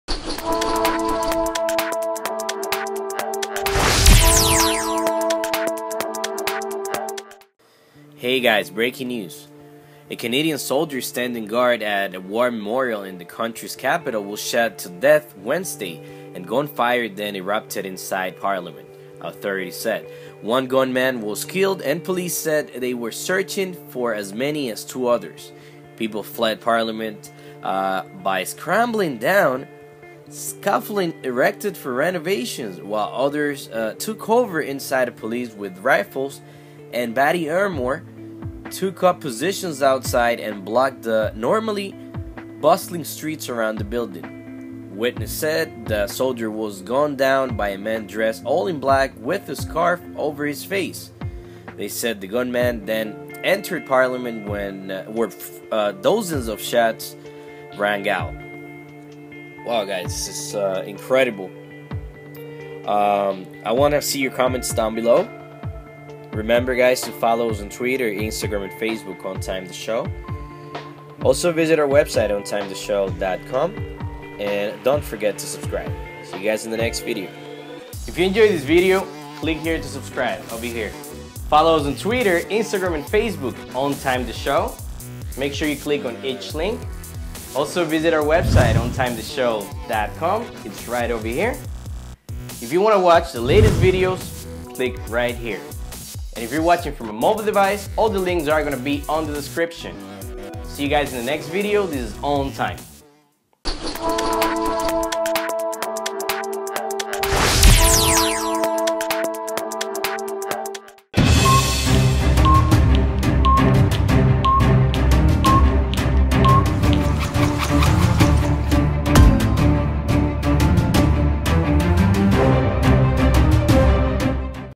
Canada Parliament Shooting - gunman shooter kills soldier inside Ottawa Parliament
Canada Parliament Shooting - gunman shooter kills soldier inside Ottawa Parliament A Canadian soldier standing guard at a war memorial in the country's capital was shot to death Wednesday, and gunfire then erupted inside Parliament, authorities said. One gunman was killed, and police said they were searching for as many as two others.

People fled Parliament by scrambling down scaffolding erected for renovations, while others took cover inside as police with rifles and body armor took up positions outside and blocked the normally bustling streets around the building.

Witnesses said the soldier was gunned down by a man dressed all in black with a scarf over his face.